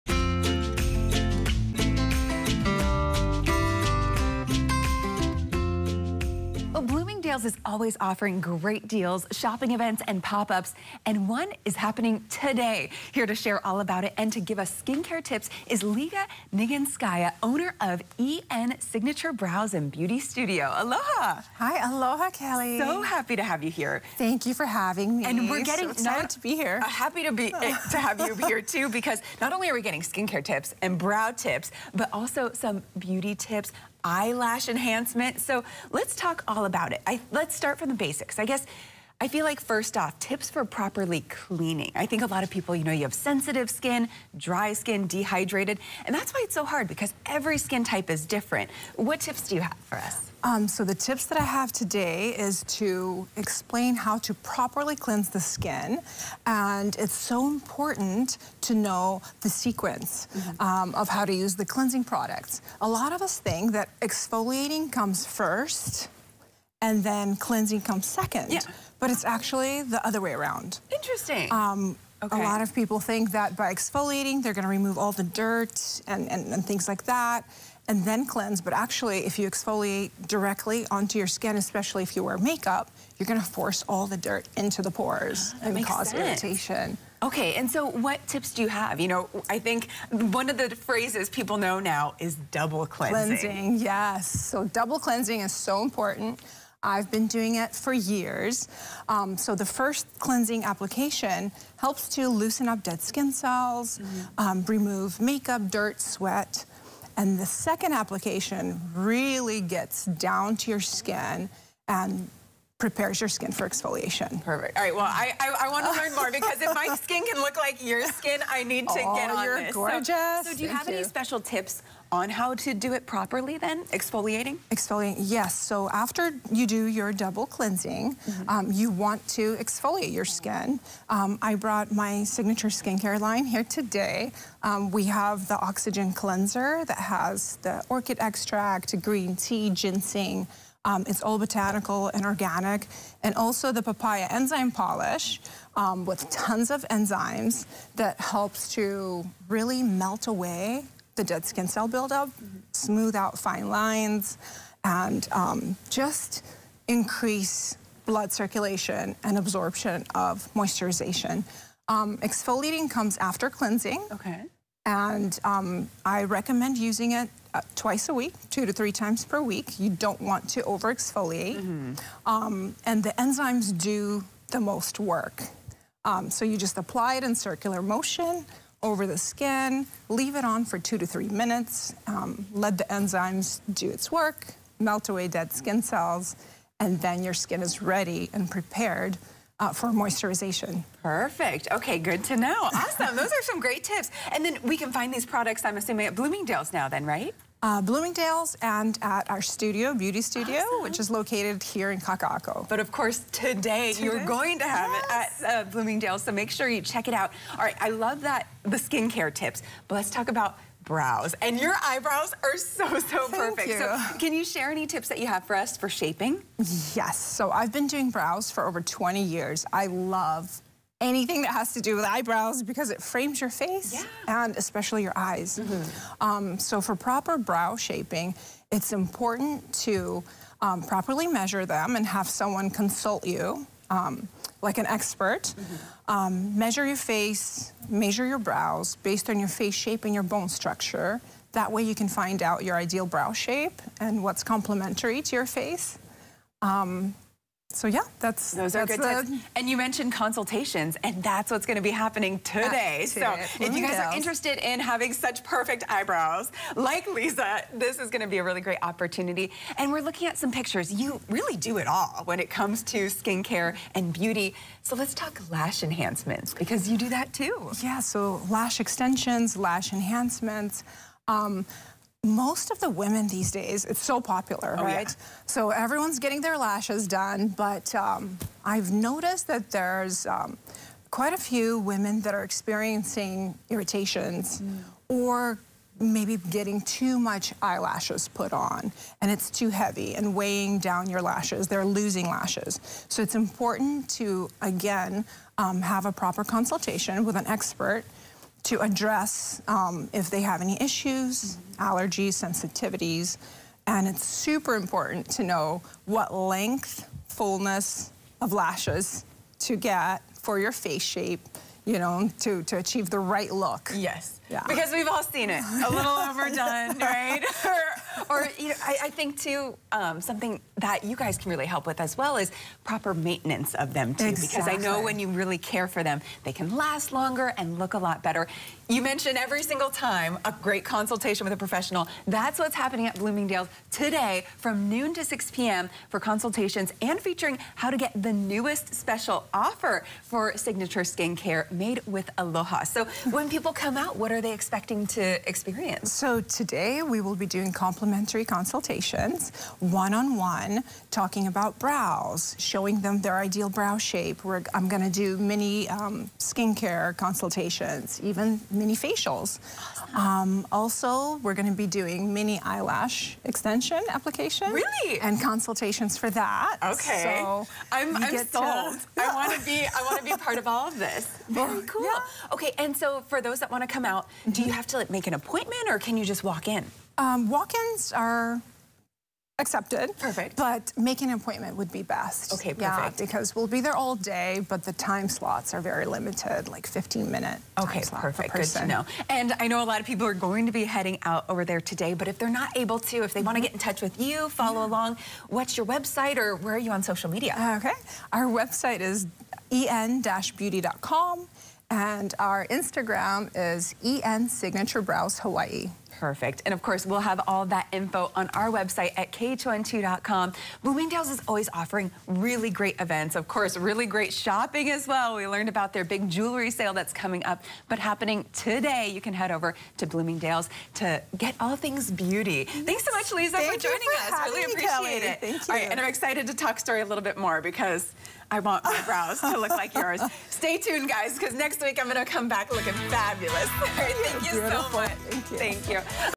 Skincare Tips at Bloomingdale's Collab Event
Bloomingdale’s Ala Moana has a beauty collab happening today! Liza Neginsky, owner of EN Signature Brows and Beauty Salon, is set to make a special appearance at Bloomingdale's on February 29th.